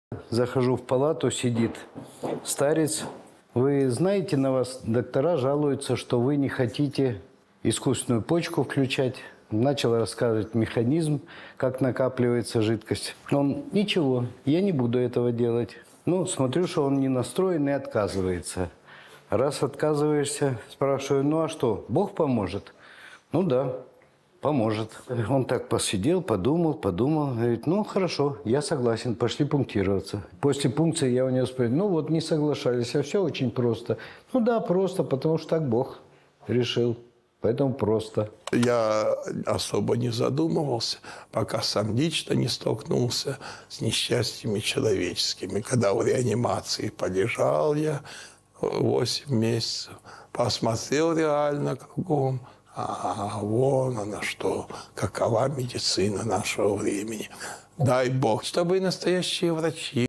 О. Зосима и доктора
С 12 марта 2022 года Свято-Успенский Николо-Васильевский монастырь, на Донбассе, находится на линии фронта, под постоянными обстрелами.
Много зданий разрушено, часть насельников, дом милосердия и все беженцы эвакуированы.
Оставшиеся братья и сестры, уповая на милосердие Божие, заступничество Пресвятой Богородицы, молитвы основателя монастыря, схиархимандита Зосимы, продолжают своё служение в стенах родной многострадальной обители.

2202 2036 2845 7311 - Сбербанк,
(Юлия Сергеевна Г.)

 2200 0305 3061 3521 - банк ПСБ, (Юлия Сергеевна Г.)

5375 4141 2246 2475 - Монобанк (Олег М.)